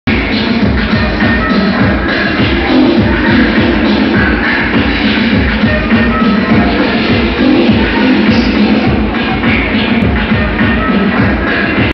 stick man dances to the music
effect designed by LIGHTNING ROD from BUBBLE VISION consisting of a trick with 4 slide projectors.the man dances to the beat of the music